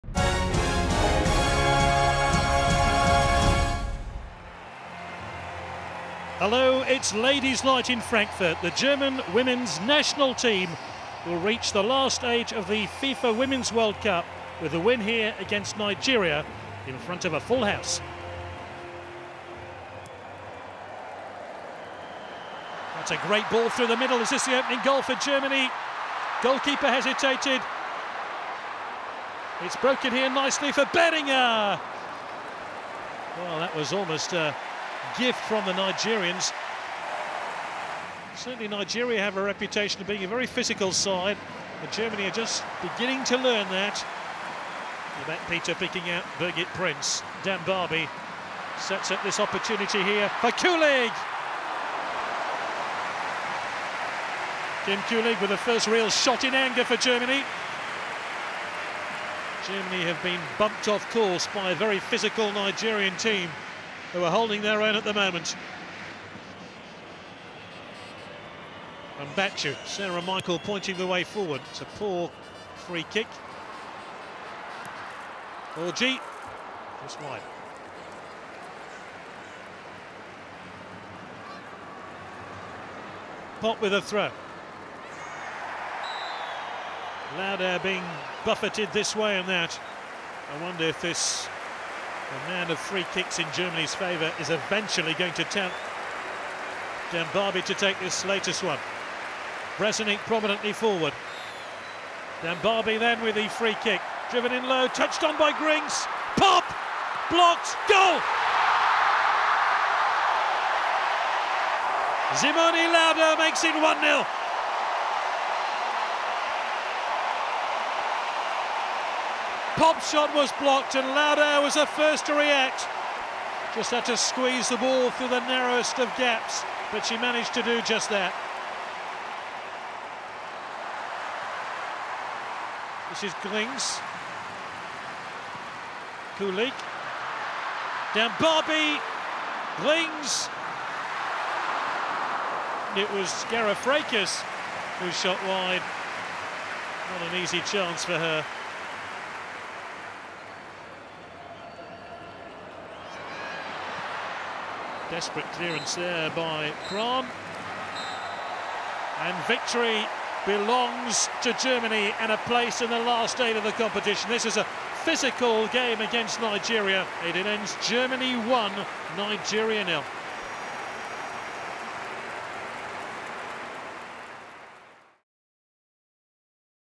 German joy at feisty winner
Germany - Nigeria, FIFA Women's World Cup Germany 2011: In a do-or-die match, Simone Laudehr eased the hosts' nerves with a scrambled goal in this hard-fought match.

FIFA Women's World Cup Germany 2011™, Frankfurt/Main, FIFA Women´s World Cup Stadium, Group A, 30 June 2011. Full match details: